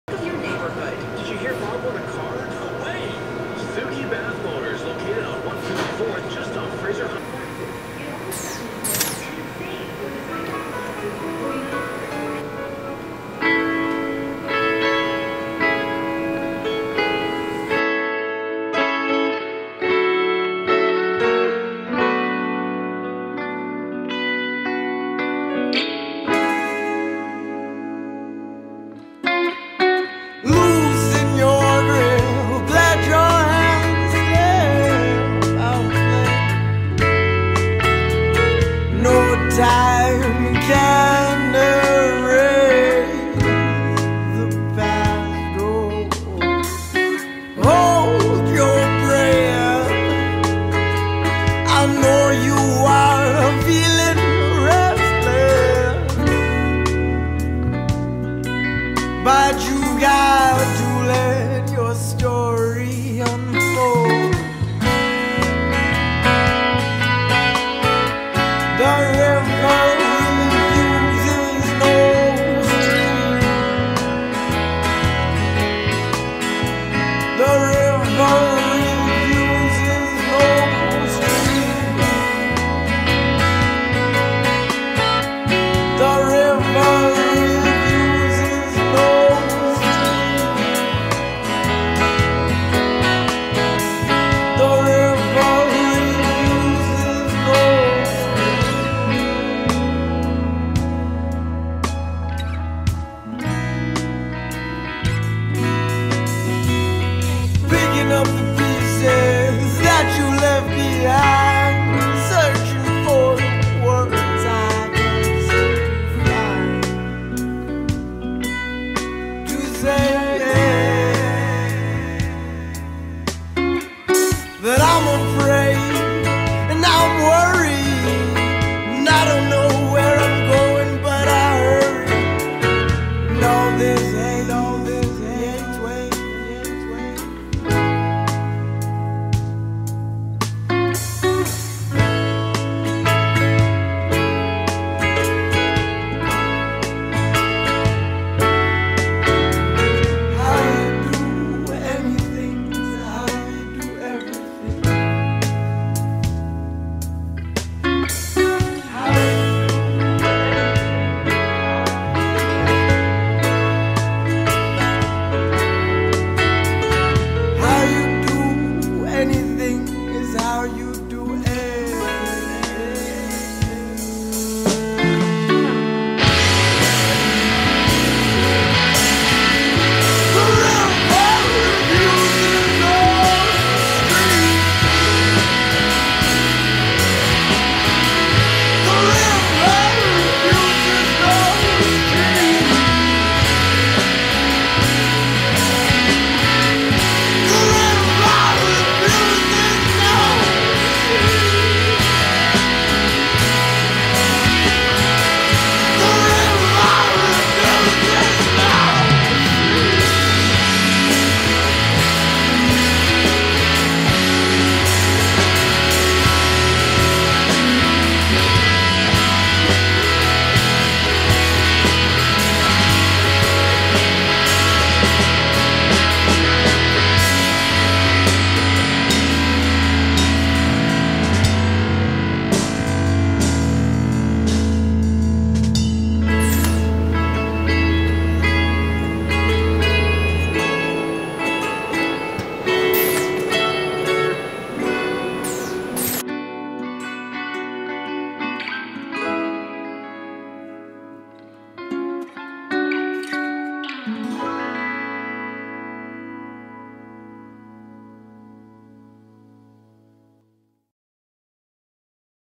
4 Colour Shirt Design ACTION By Katie Hodgson (feat. Lambsbreath)
I've moved to a new shop in Vancouver and we did this shirt together! I've been throwing these shirts in with my clients' orders since we printed them, and was super stoked to see them printed!

And give Lambsbreath a listen! They recently won our local Band Fight at Joe's Apartment in Vancouver, and we'll be collaborating on more videos, album art, and probably shirts in the future.